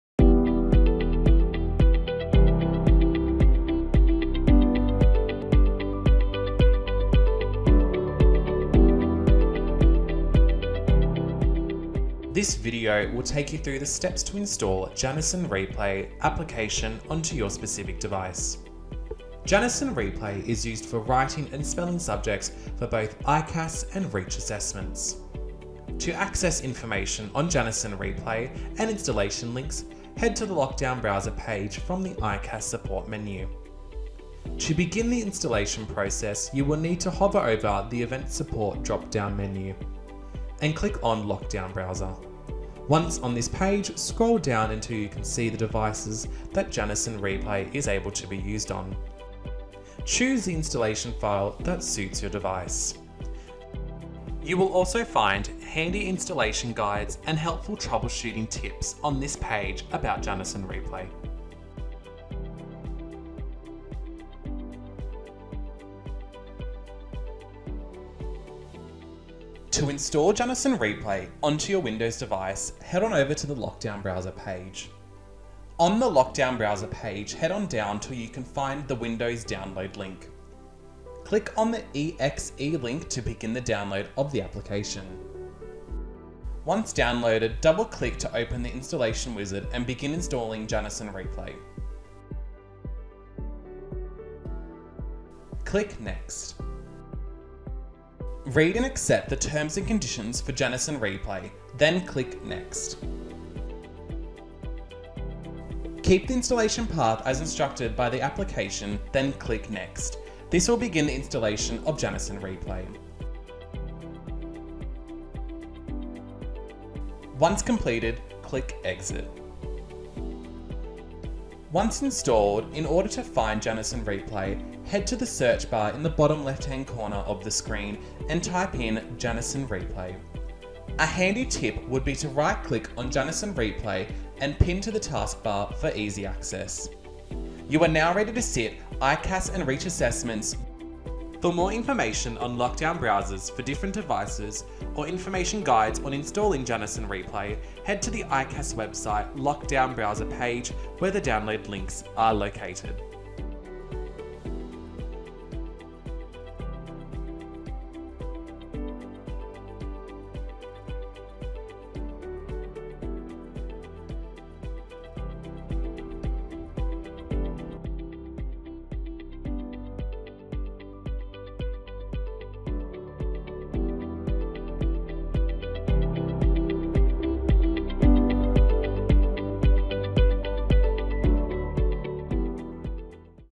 ICAS Assessments: Learn how to install the locked-down Janison Replay browser on an Windows device
Learn how to install Janison Replay onto the locked-down browser on your Windows device. Janison Replay is used for Writing and Spelling subjects for both ICAS and Reach Assessments.